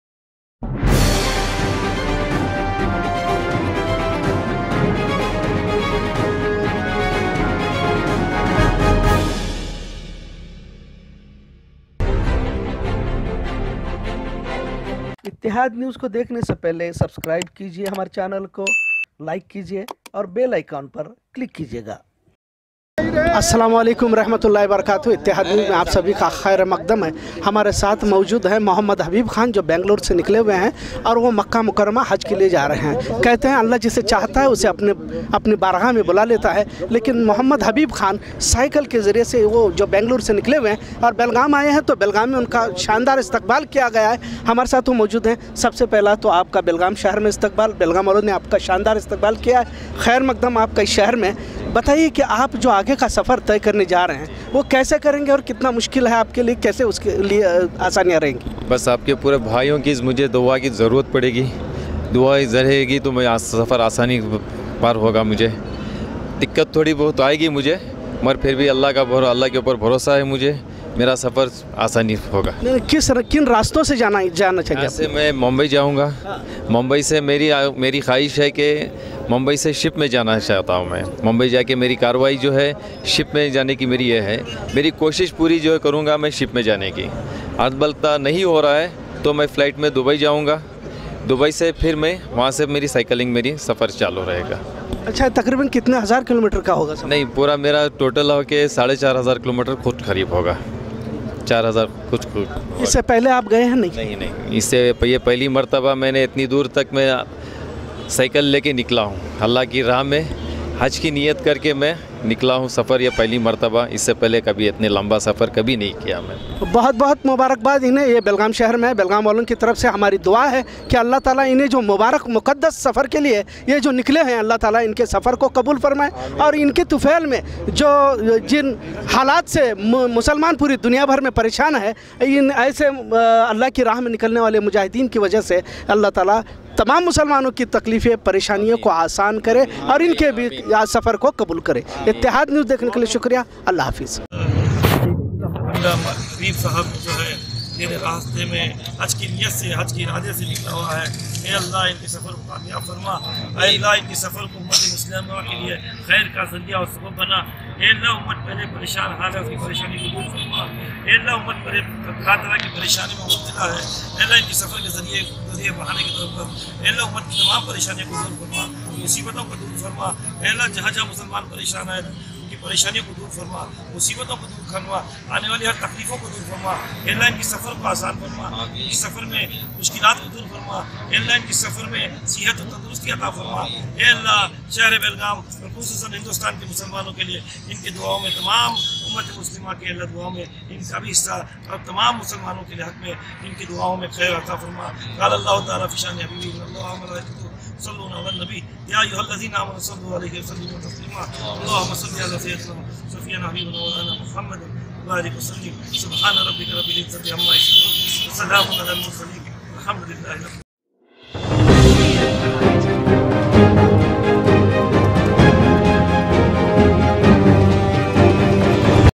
ITTEHAD NEWS | BELGAUM NEWS | - BANGALORE TO MAKKAH CYCLING ON HAJJ
BANGALORE TO MAKKAH CYCLING ON HAJJ